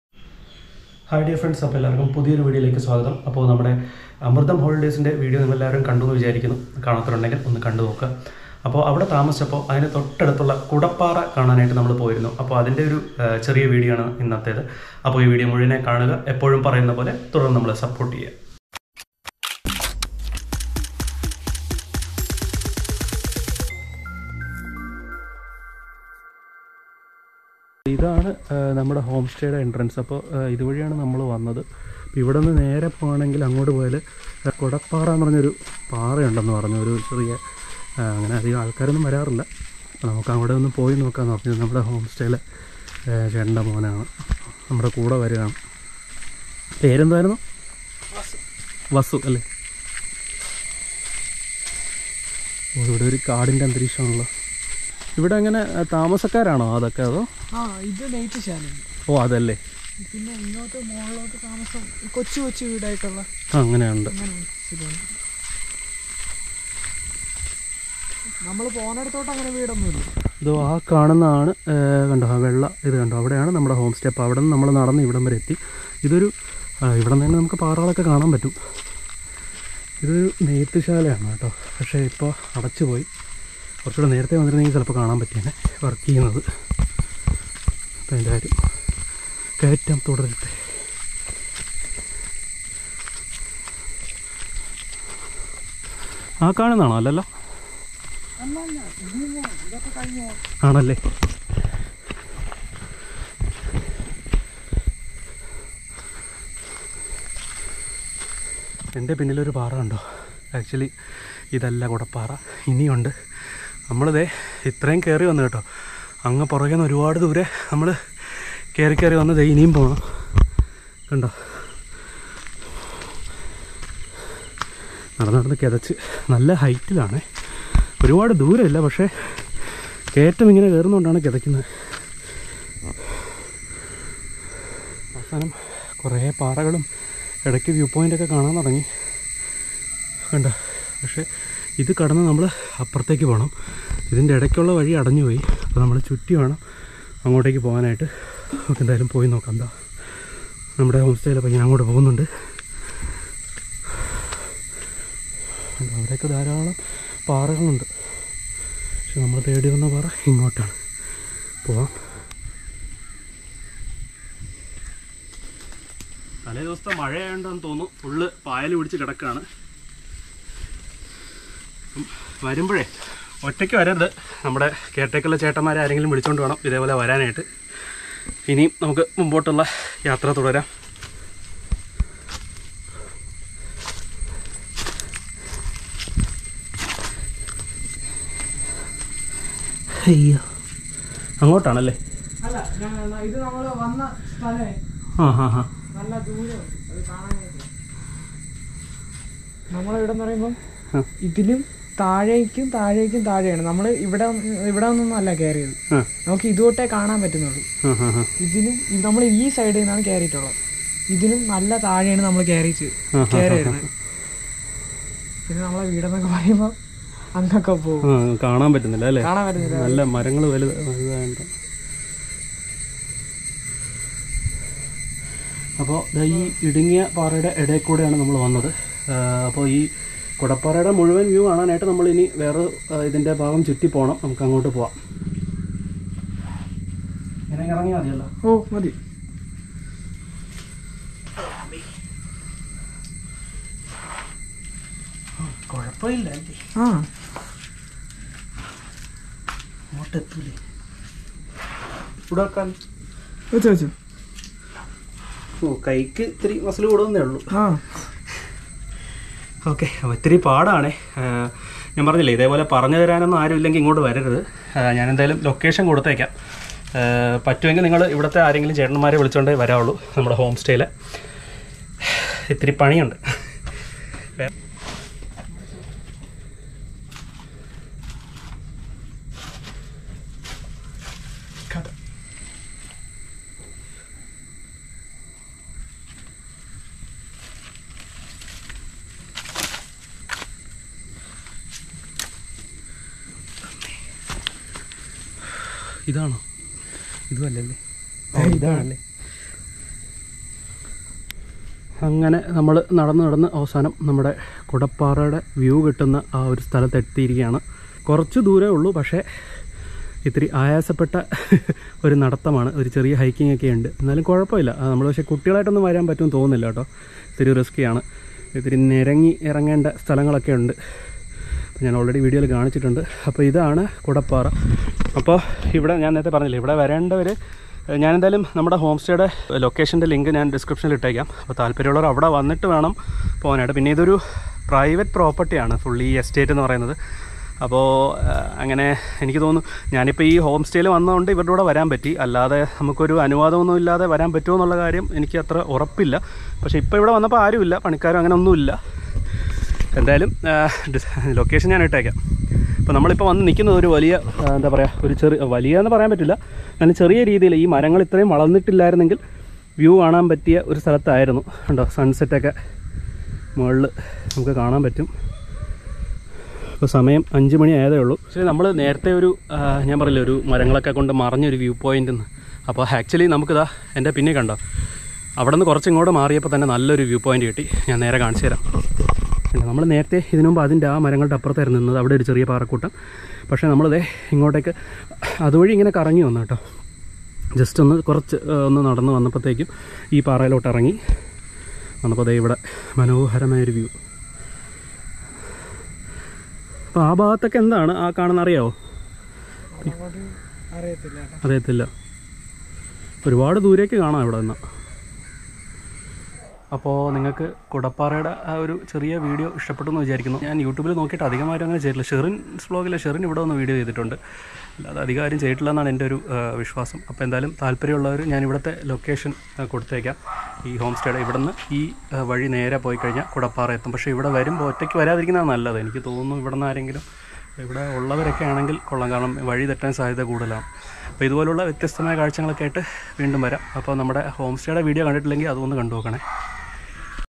Kudappara - Another Hill top in Trivandrum | Places to visit in Trivandrum
This weekend, we would like to show a place called Kudappara. This is an unexplored location where you can find a cave and a beautiful view point.